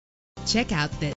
2012 Chevrolet Impala - Foley AL
Year: 2012
Make: Chevrolet
Model: Impala
Trim: LT
Engine: 3.6 liter 6 cylinder 24 valve
Transmission: 6-Speed Automatic
Color: Silver
Mileage: 41543
Address:
2255 S Mckenzie St